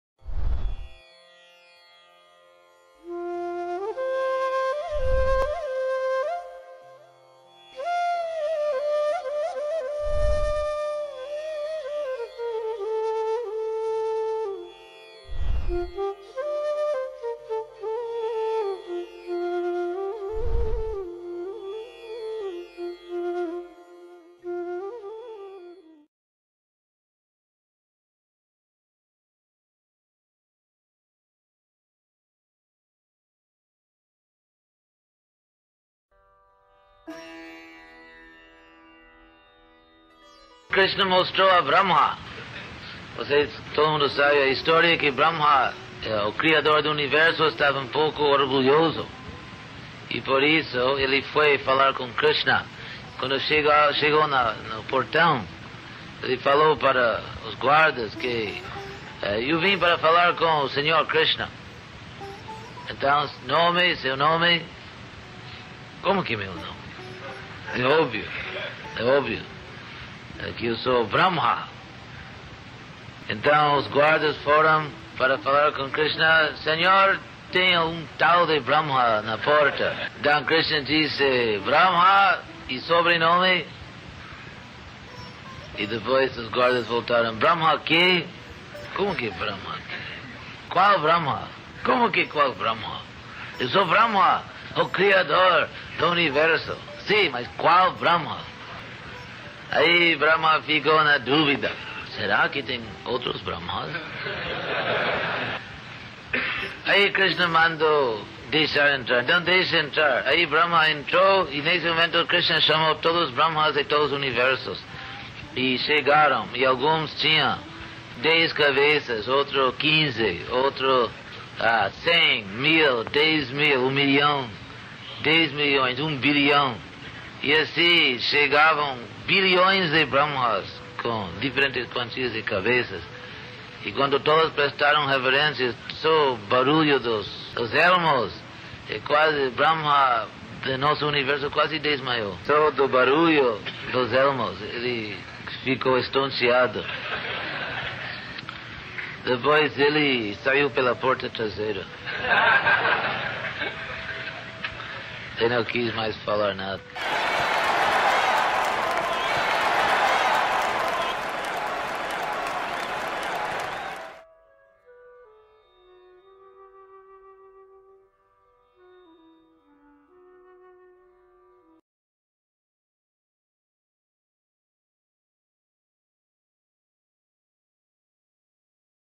Acharyadeva conta a história: Senhor Brahma vai até Dwaraka
Em uma palestra dada no Brasil, Hridayananda Dasa Goswami Acaryadeva conta a história em que o Senhor Brahma foi até Dwaraka orgulhoso de sua posição neste universo e Krishna o faz mais humilde.